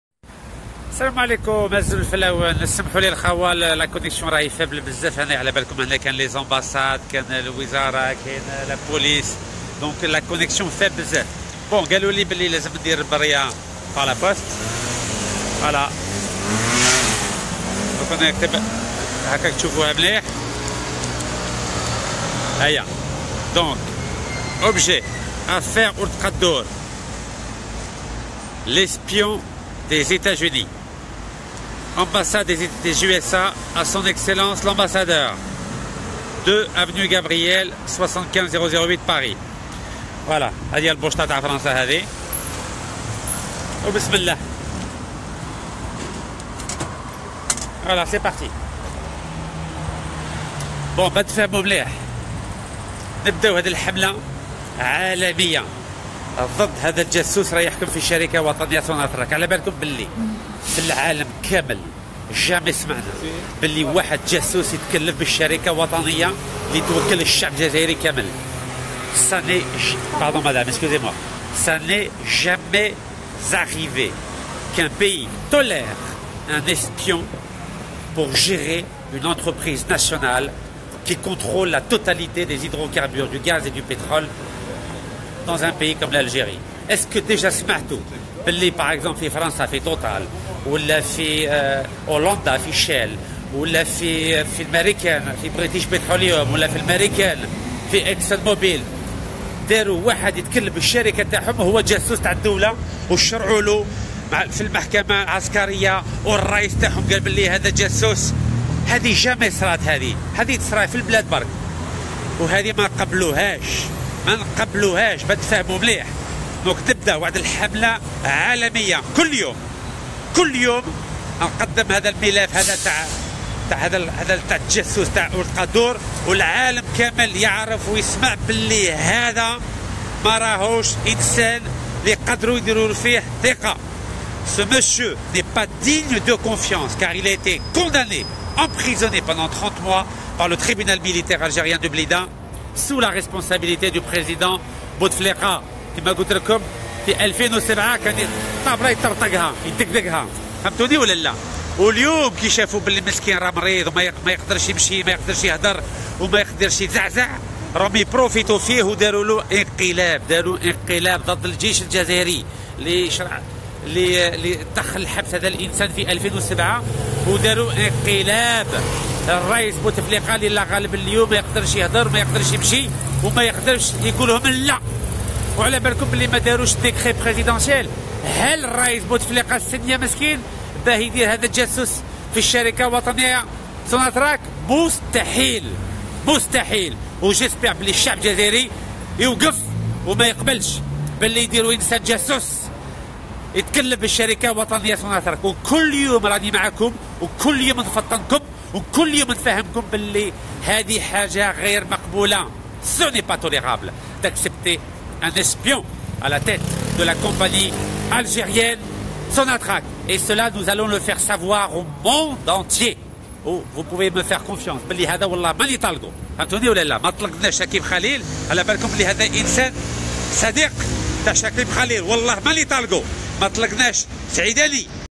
رشيد نكاز يعلن بداية حملته العالمية ضد الجاسوس ولد قدور الذي يحكم سوناطراك جزء2
OPÉRATION " NO SPY IN ALGERIA" : Après le coup d'Etat contre l'armée et contre le president Bouteflika qui ont jugé cet espion en 2007, Rachid Nekkaz commence la tournée des 195 ambassades mondiales afin d'obtenir la DEMISSION de la SONATRACH de l'espion des Américains OULD KADDOUR.

وقعوا هنا من فضلكم لدعم حركة الشباب والتغيير

RACHID NEKKAZ et Le MJC EXIGENT LA DEMISSION, DE LA SONATRACH, DE L'ESPION OULD KADDOUR, l'AMI DU VOLEUR CHAKIB KHELIL